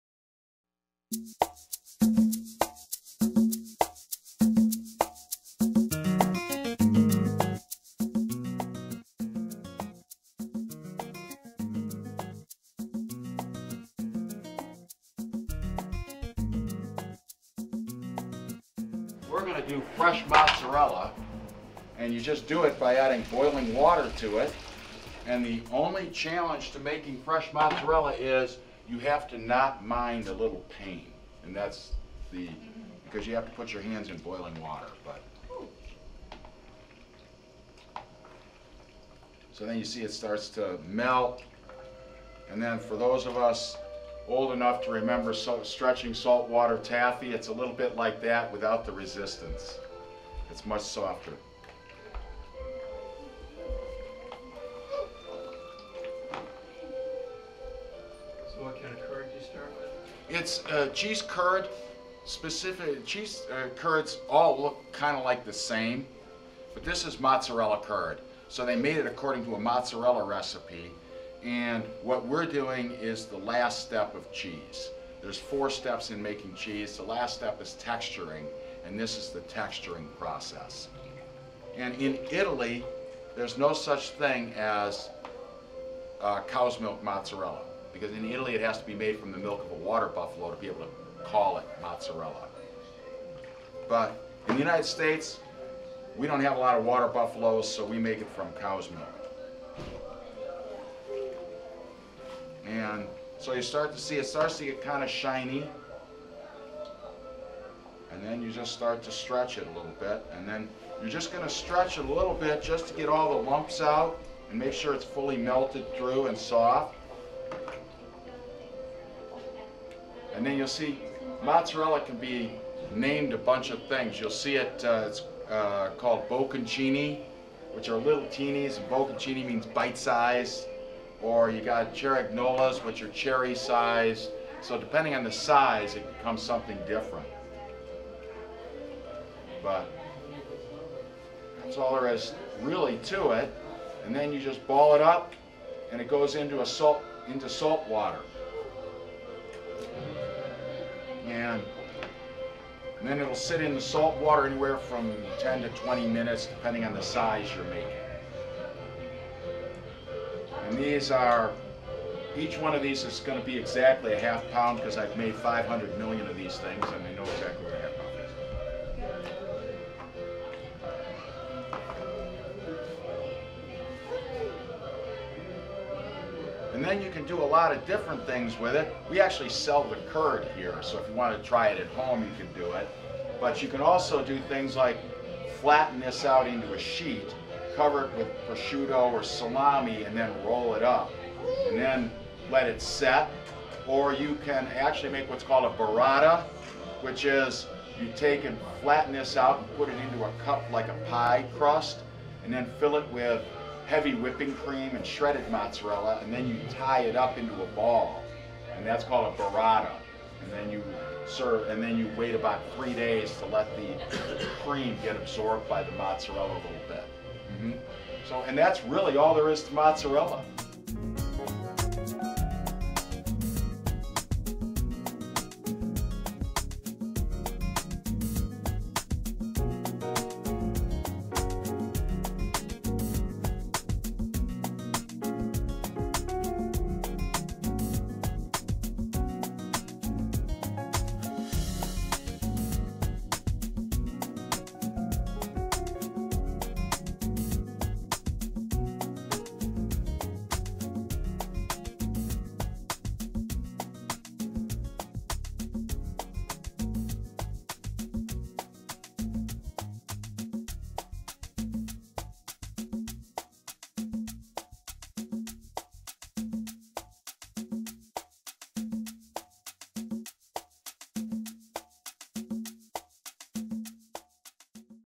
Saline Winterfest 2018 - Cheese Making at the Saline Cheese Shop
Part of the 2018 Saline Main Street Winterfest